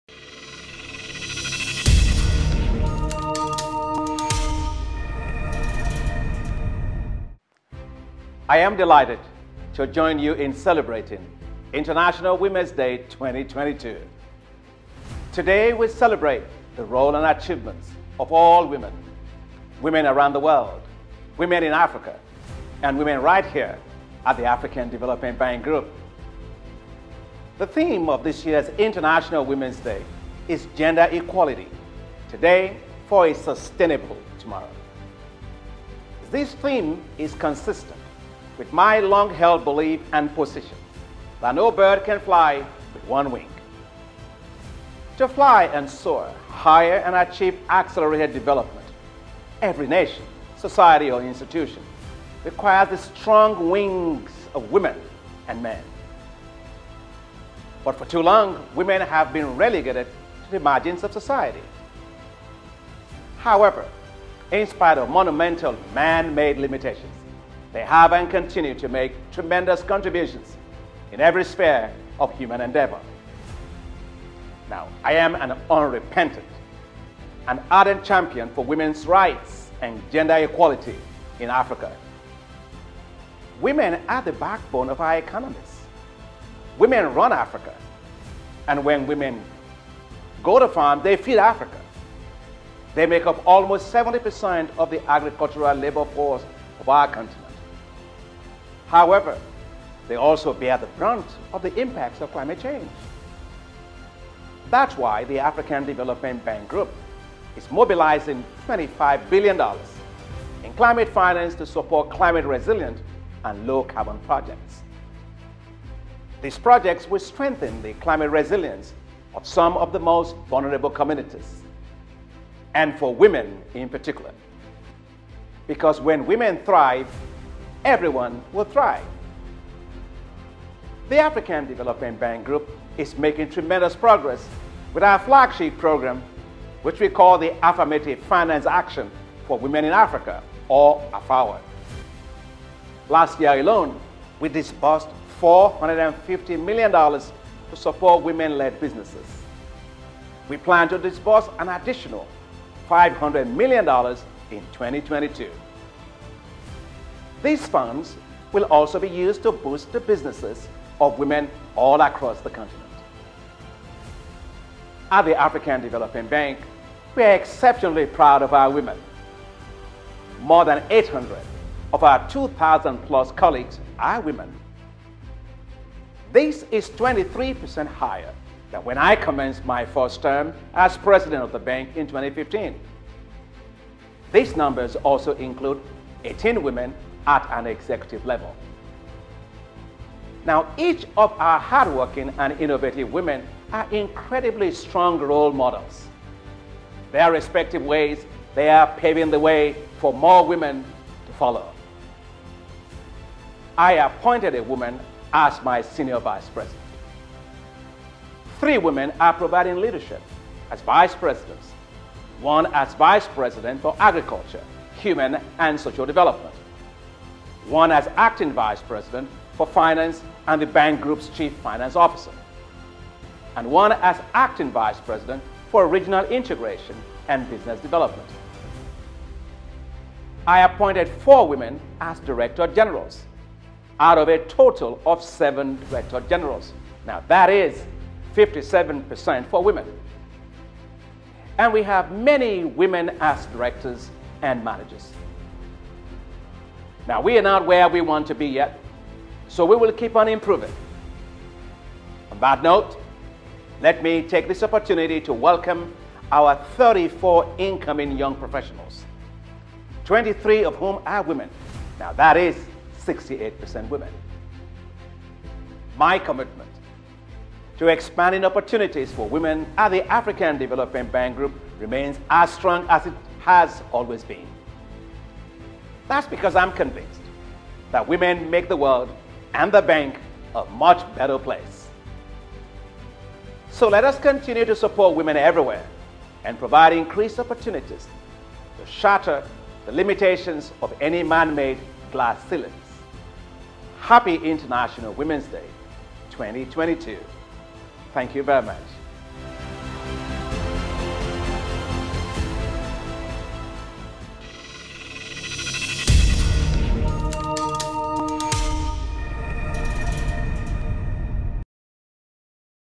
AfDB President Adesina’s wishes on International Women’s Day 2022
To mark International Women’s Day, celebrated annually on 8 March, African Development Bank Group President, Dr. Akinwumi A. Adesina, shares a message pledging his continued dedication to the economic success of African women.

The theme of this year’s celebration is “Gender Equality Today for a Sustainable Tomorrow.”

Watch this video to learn more about the African Development Bank’s commitment to women, including the number of women holding senior management positions at the institution and the Bank’s investments to women-led businesses through its Affirmative Finance Action for Women in Africa (AFAWA) initiative – which in 2021 alone, dispersed $450 million to women entrepreneurs.